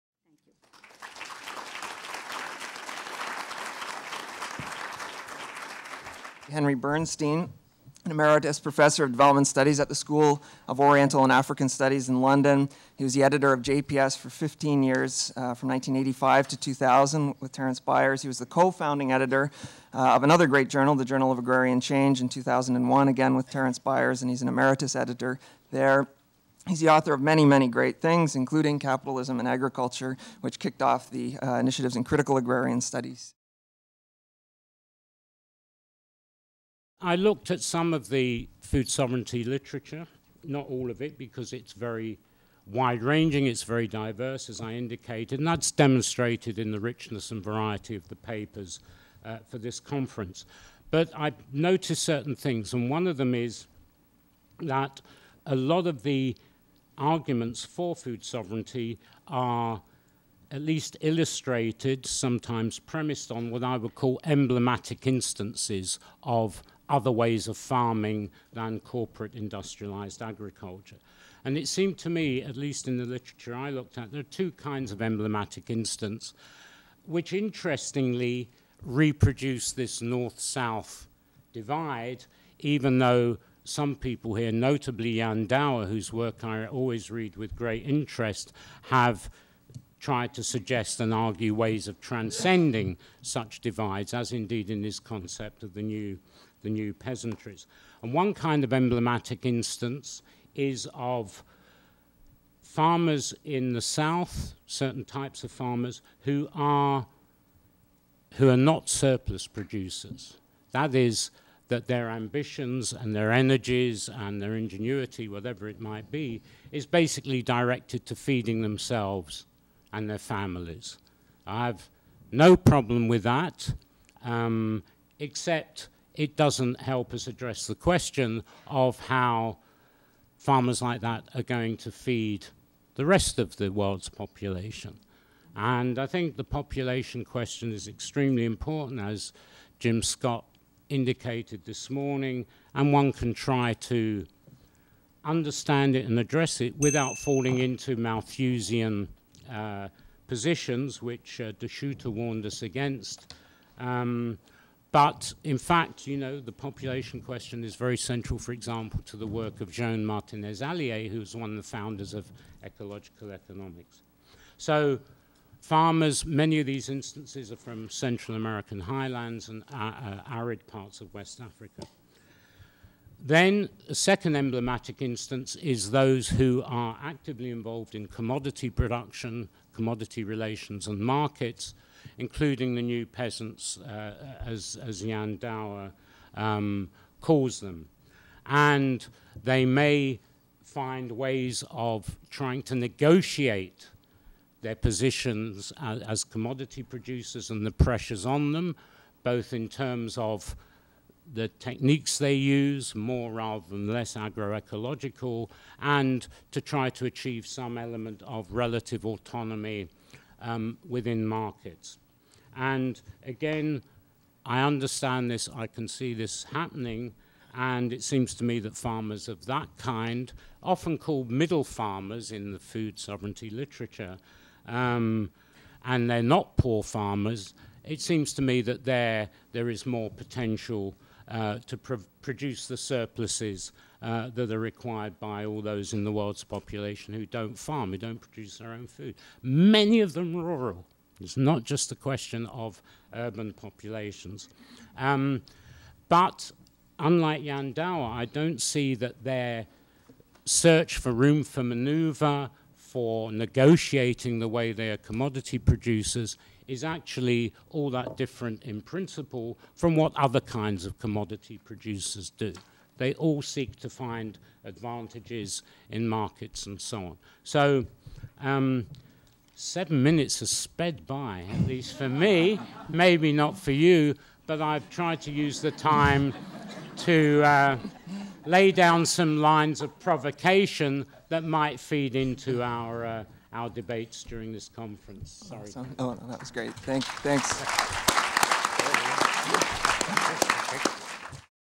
Henry Bernstein: Food sovereignty A critical dialogue
Food sovereignty: a skeptical view

Organized by the Yale University Agrarian Studies Program and The Journal of Peasant Studies in collaboration with Food First, Initiatives in Critical Agrarian Studies/International Institute
of Social Studies (The Hague), Transnational Institute (Amsterdam), the Yale Sustainable Food Project,
and Yale South Asian Studies, with support from Kempf Fund